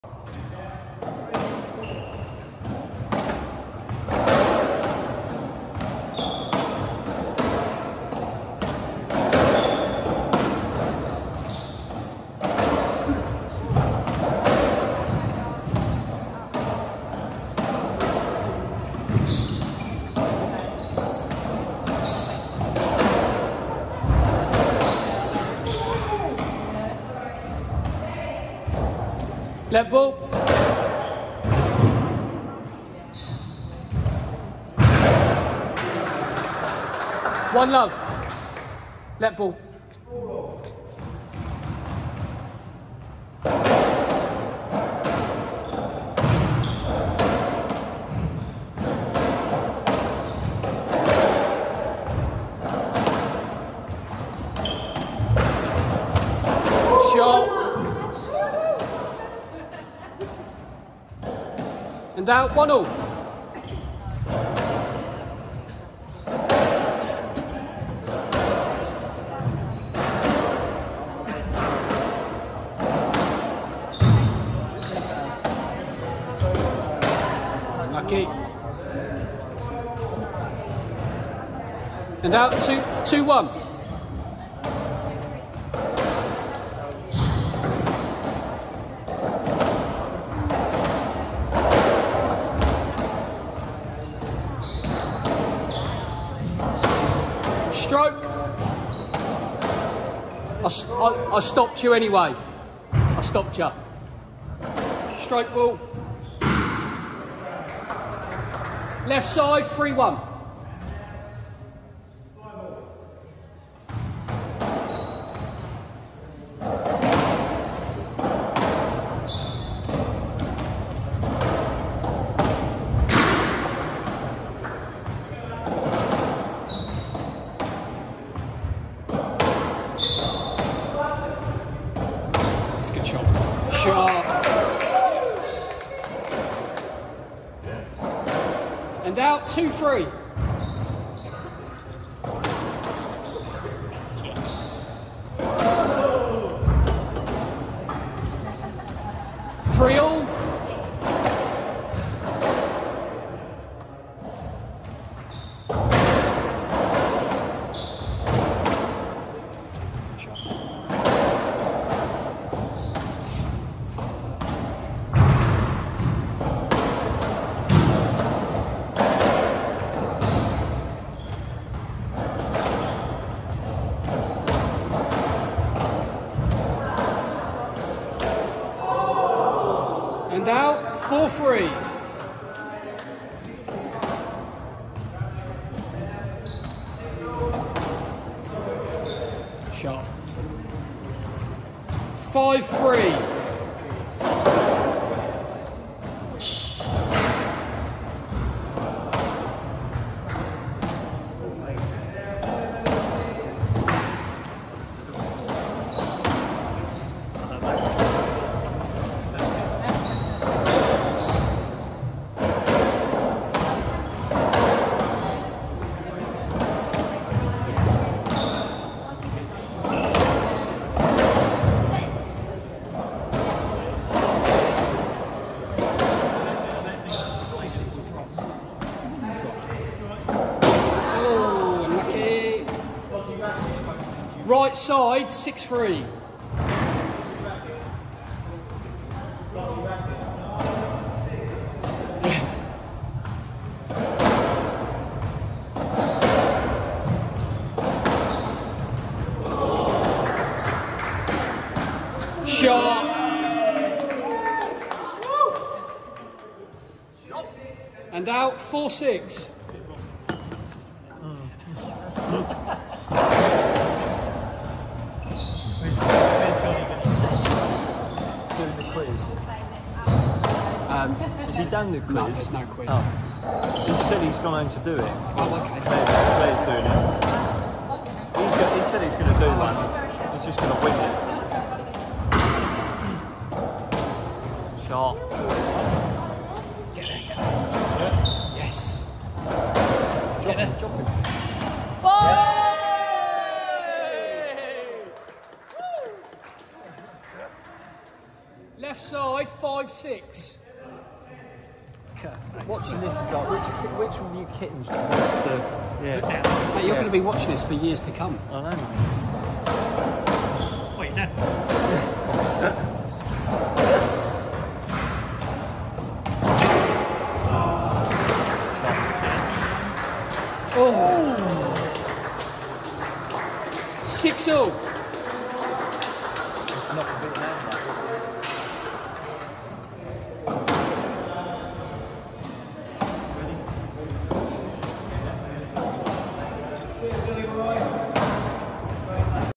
John Moore Vs Christina Moore at The Dover Racketball Open 2023 🩷💚
Mr Moore senior takes on daughter Christina Moore at The Dover Racketball Open￼ 2023 🩷💚